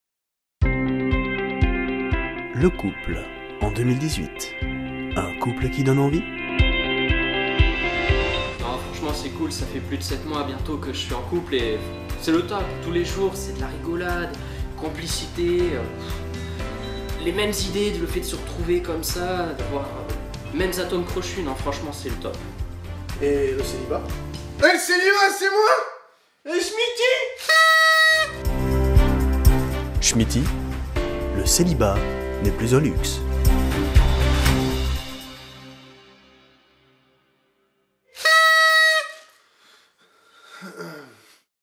Lenzorino - Schmidty
Ce n'est plus un luxe d'être célibataire en 2018 ! Et un nanar de parodie pub en plus avec cette nouvelle vidéo.. pas si nouvelle d'ailleurs !

Pour la petite anecdote : le tournage a été réalisé en décembre 2015. Oui j'ai un peu tardé je sais. Désolé !

Bon visionnage ;)